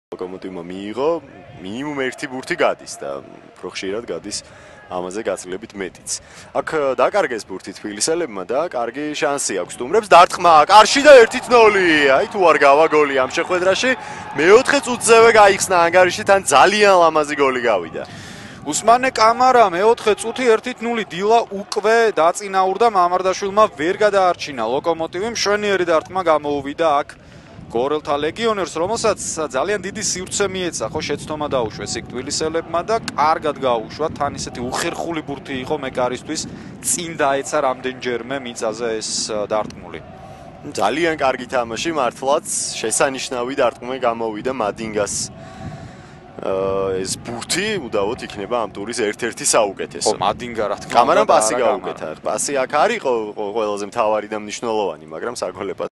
Francisco Madinga
Beautiful goal from a young talented Malawian